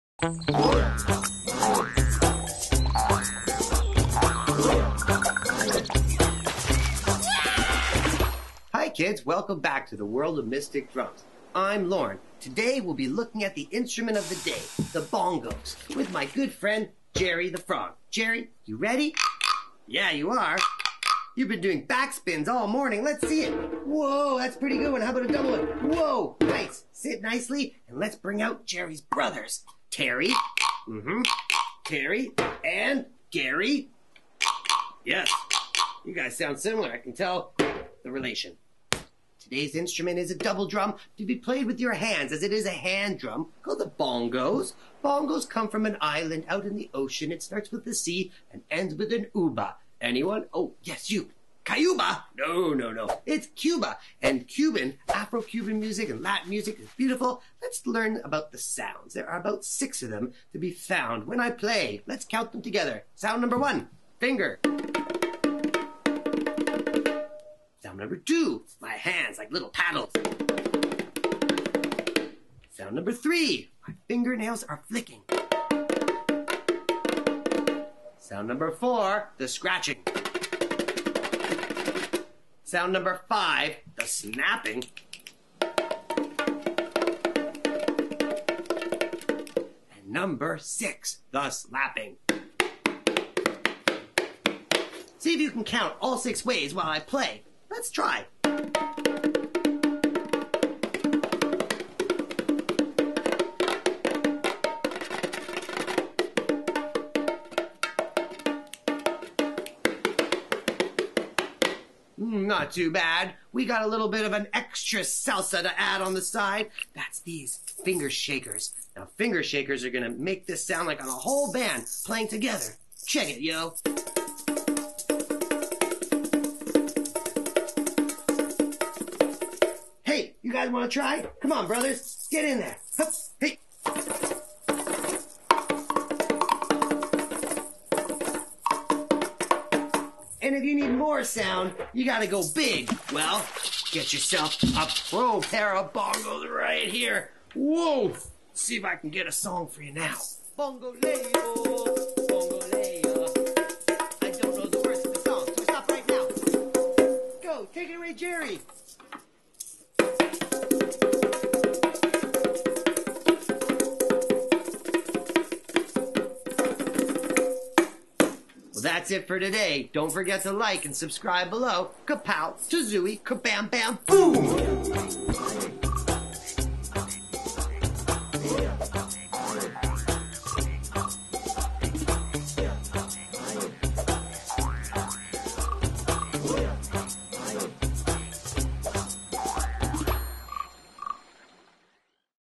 Bongos | 021 | Mystic Drumz Children's Entertainment
Lorne, Jerry, Terry, and Gary learn about Bongos!

World Music Safari Adventures with Lorne and Jerry the Frog

Episode 021: Bongos

Mystic Drumz invites children to travel the planet in search of rare and unique percussion instruments and drums!

Music Education with a new and fun approach!